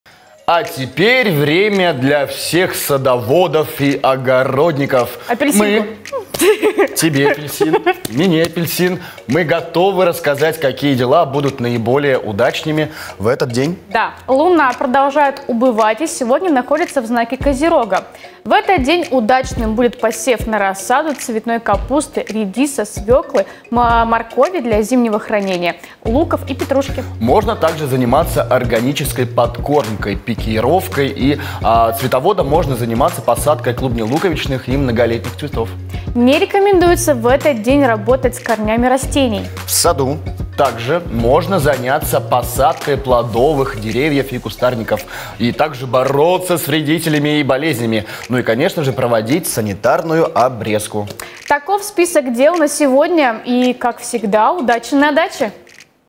Апельсин. Садовый календарь на 10 мая
Цветоводам 10 мая рекомендуется заняться посадкой клубнелуковичных и многолетних цветов. Но не стоит в этот день работать с корнями растений. Подробности – в нашей постоянной рубрике «Садовый календарь».